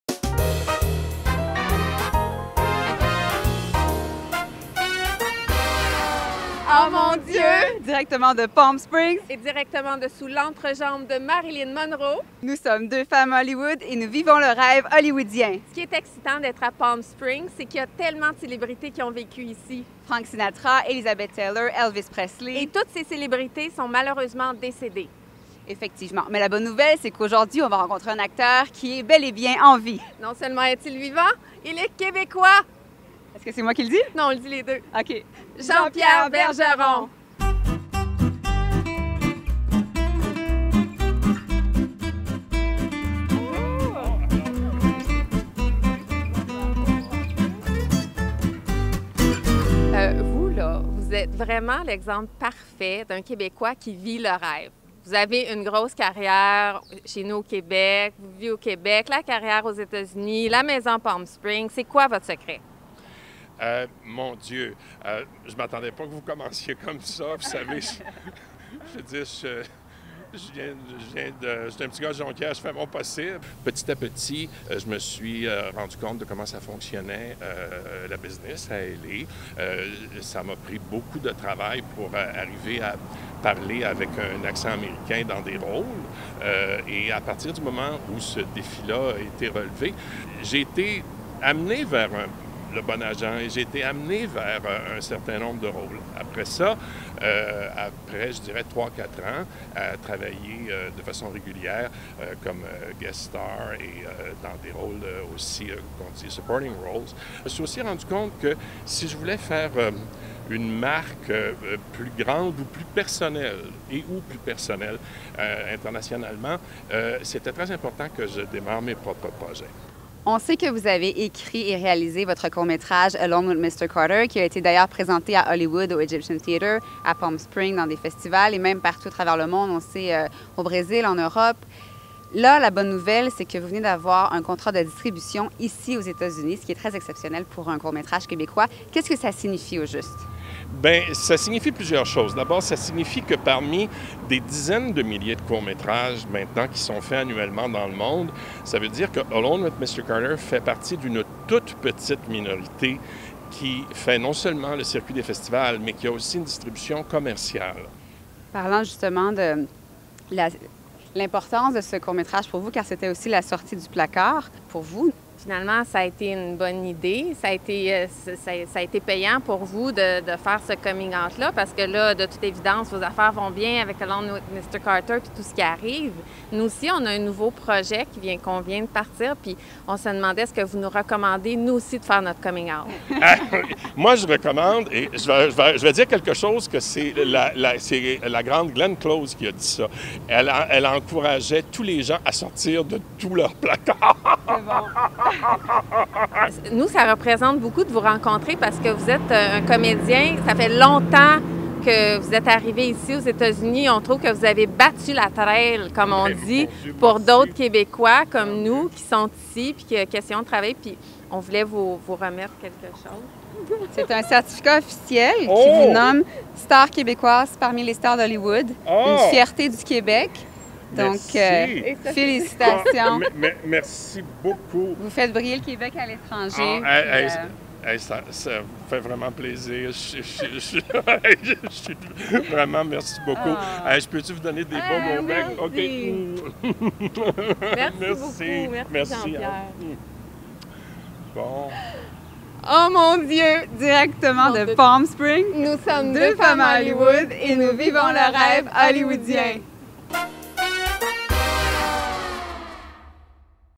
2 Femmes à Hollywood et Jean-Pierre Bergeron.
L'équipe de 2 Femmes à Hollywood a eu la chance de rencontrer Jean-Pierre Bergeron, à Palm Springs, pour couvrir le lancement de son film "Alone with Mr. Carter".

Animatrices: Sergine Dumais et Mukta Cholette
Invité: Jean-Pierre Bergeron
Réalisateur: Bo Price
Auteur: Bo Price
Camera 1: Rob Inderrieden
Camera 2: Bo Price

© 2 Femmes à Hollywood